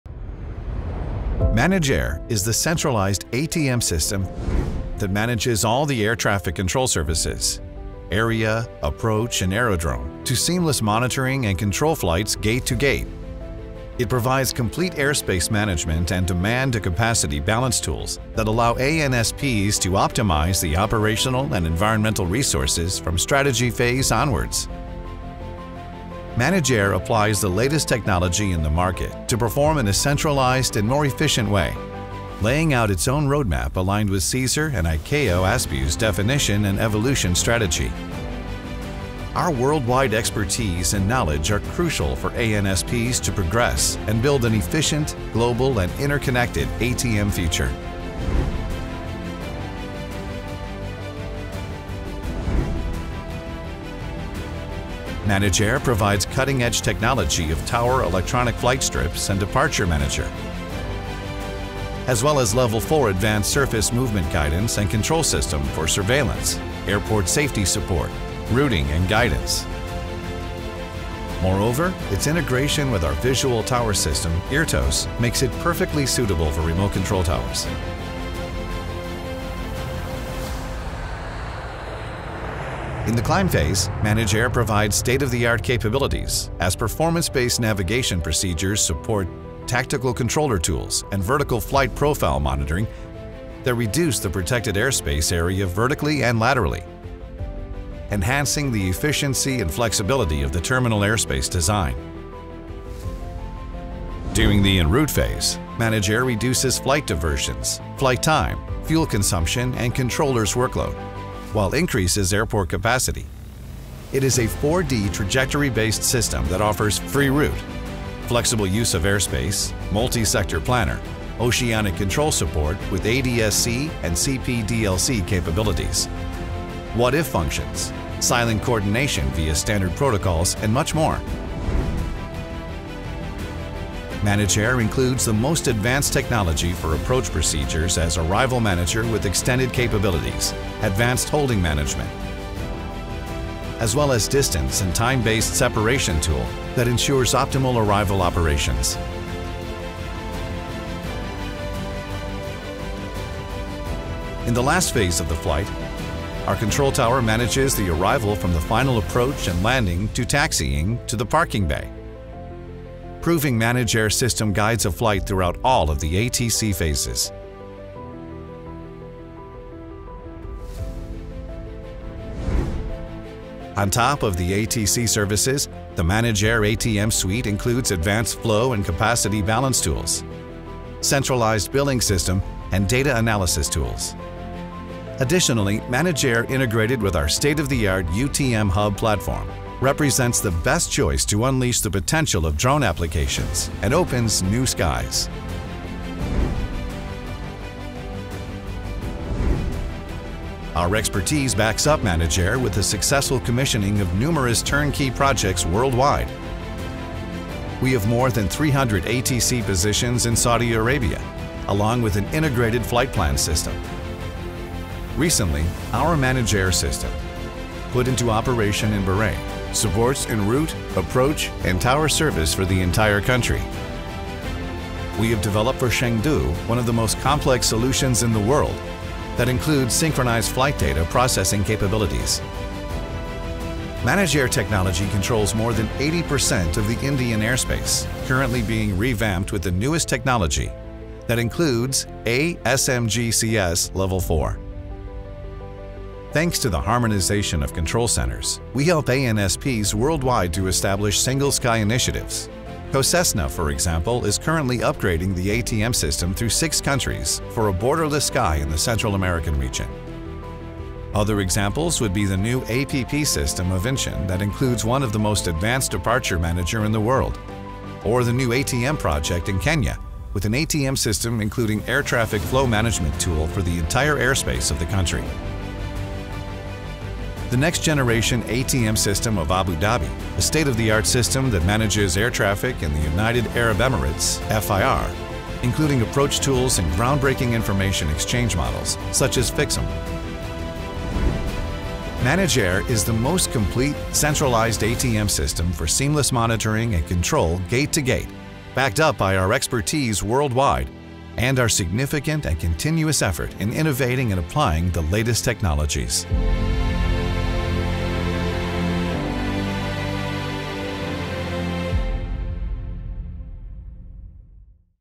ManagAIR | The centralised ATM Suite
ManagAir is the centralised ATM system that manages all the air traffic control services – area, approach and aerodrome, to seamless monitoring and control flights gate to gate.
It provides complete airspace management and demand and capacity balance tools that allow ANSPs to optimise the operational and environmental resources from strategy phase onwards.

ManagAir applies the latest technology in the market to perform in a centralised and more efficient way, laying out its own roadmap aligned with SESAR and ICAO ASBUs definition and evolution strategy.

Our worldwide expertise and knowledge are crucial for ANSPs to progress and build an efficient, global and interconnected ATM future.